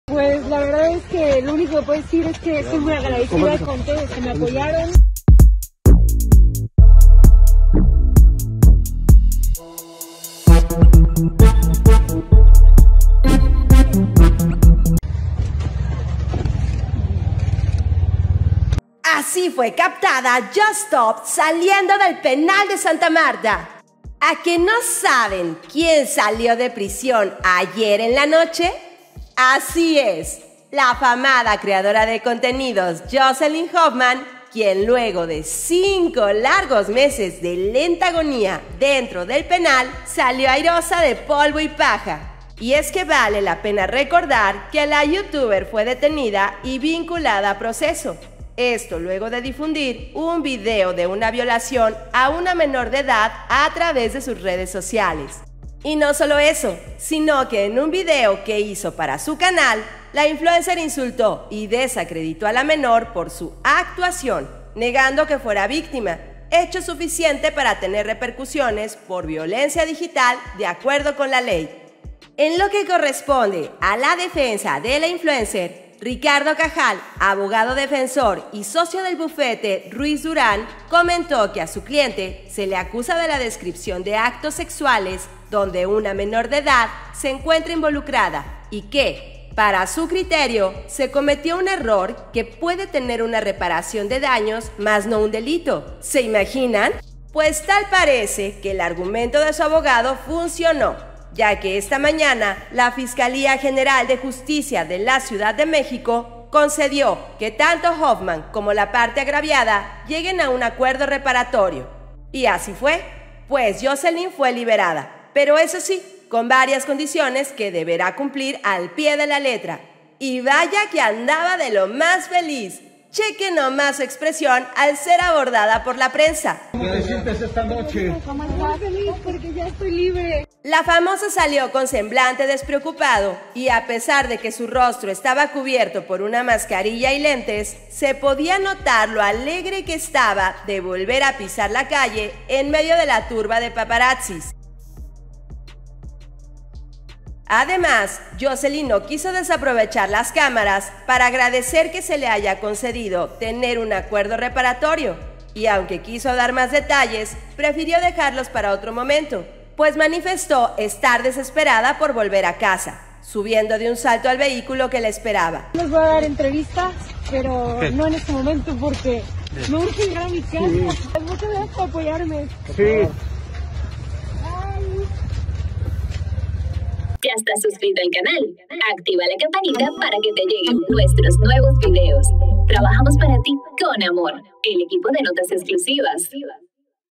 Así fue captada YosStop saliendo del penal de Santa Martha!
La influencer habló con la prensa al momento de su salida.

MFL NETWORKS INC.